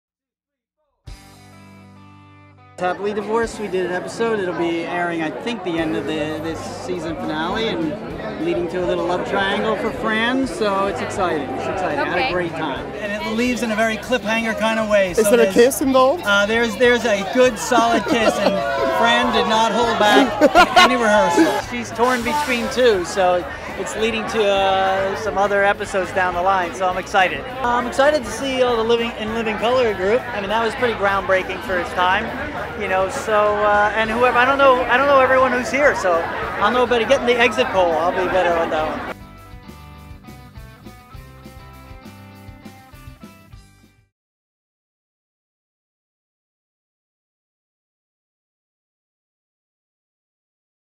2012 TV Land Awards: Karate Kid Ralph Macchio
Ralph Macchio walked the red carpet at the 10th Anniversary of the TV Land Awards. Ralph talks about working on Happily Divorced with Fran Drescher! Tune in Sunday, April 29 at 9PM/8C!